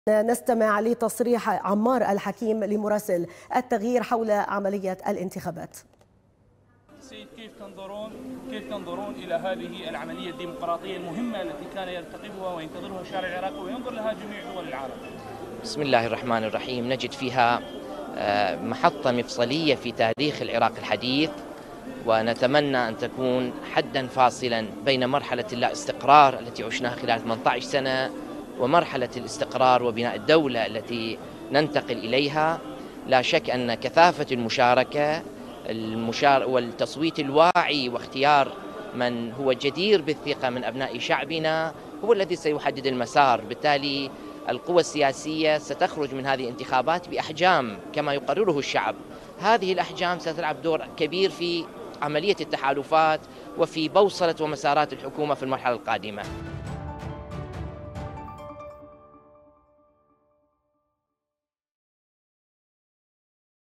عمار الحكيم للتغيير: انتخابات تشرين محطة مفصلية في تاريخ العراق
عمار_الحكيم للتغيير: التصويت الواعي للشعب سيحدد مسار الحكومات القادمة